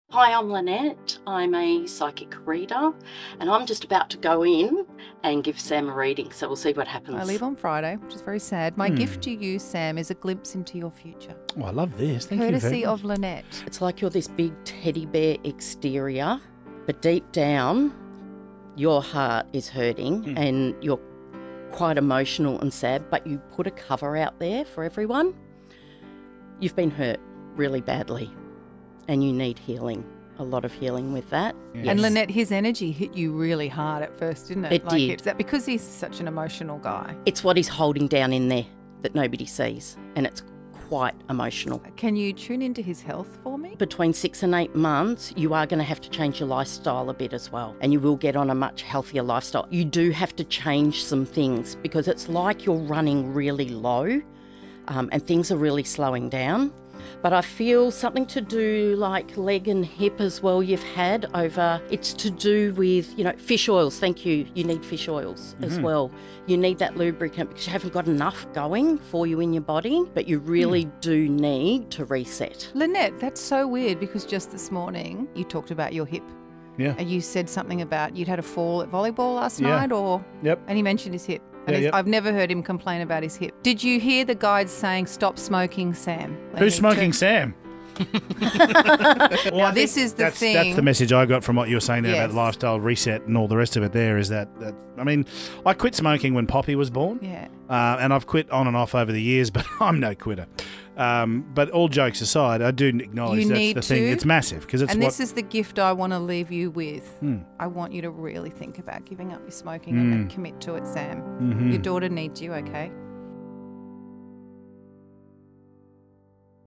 Your Heart is Hurting
"Your Heart is Hurting" - Sam's Psychic reading hit some hard truths 😢